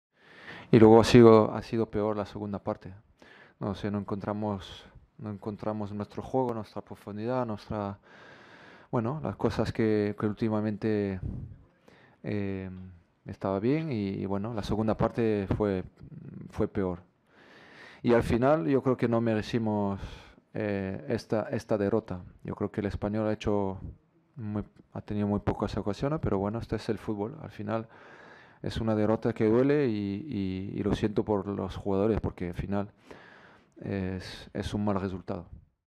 Zidane 'hurt' after last-gasp defeat to Espanyol
After a shock last-gasp defeat to Espanyol in La Liga, Real Madrid coach Zinedine Zidane said the loss was painful and that their performance didn't merit a win.